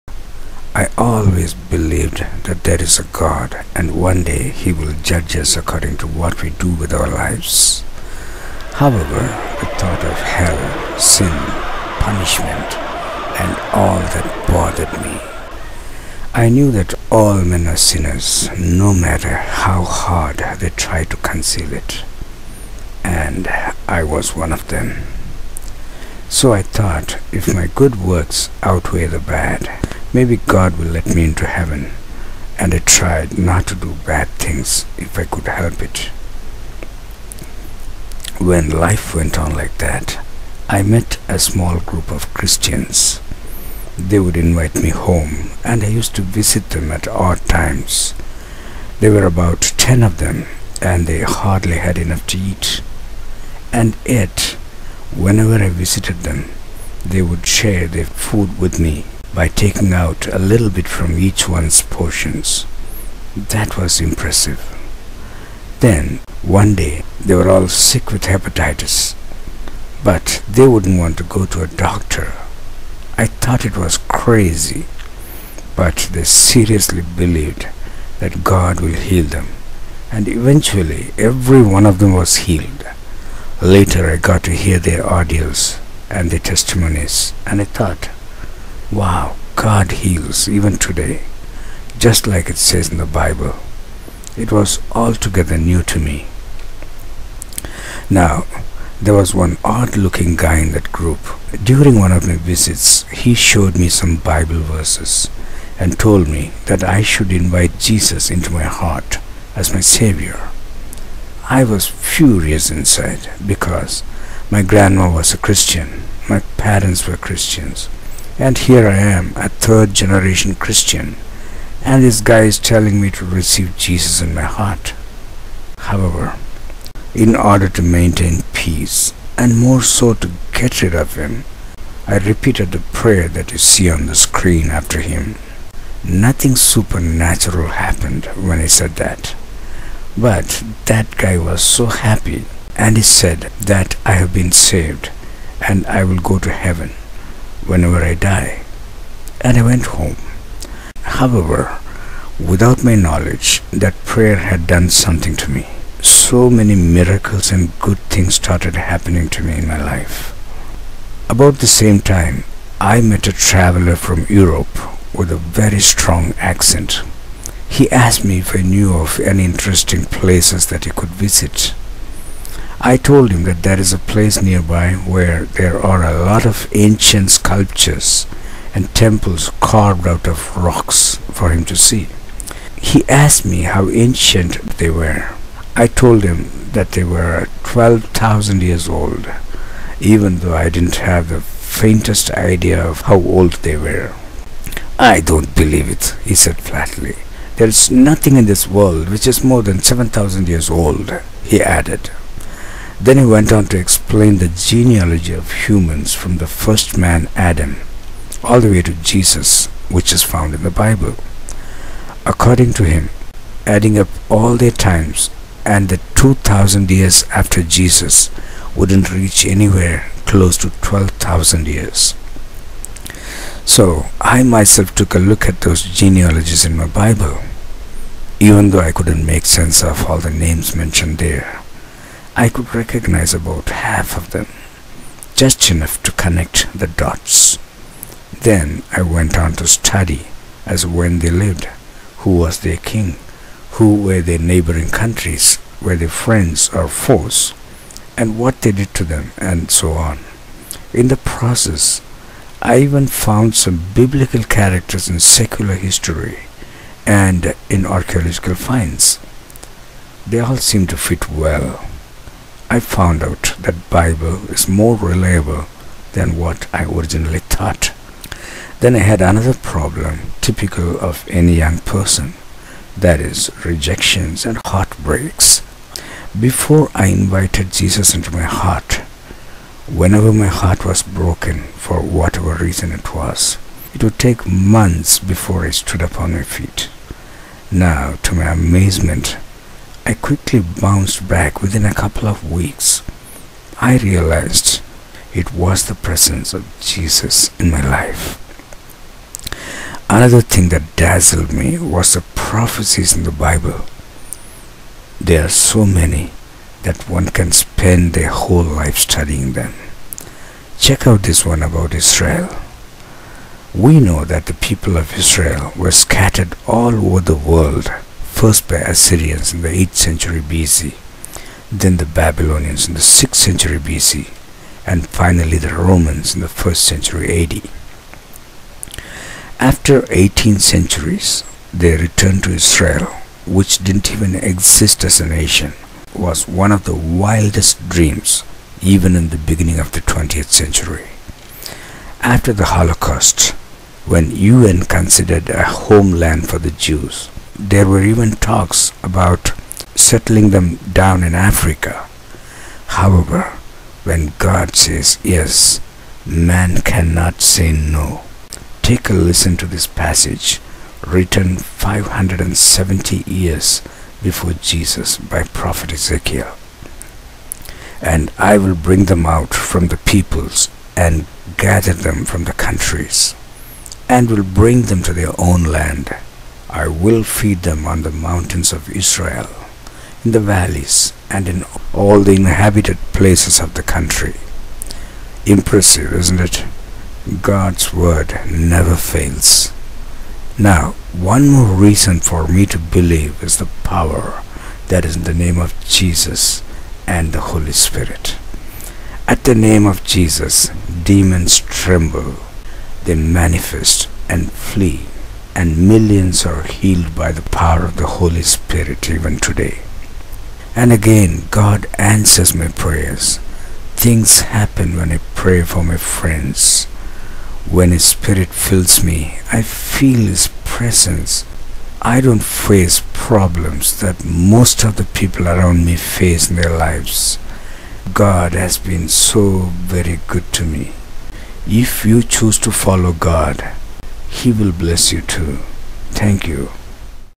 WHY DO I BELIEVE?
This is my personal testimony of faith in Lord Jesus Christ...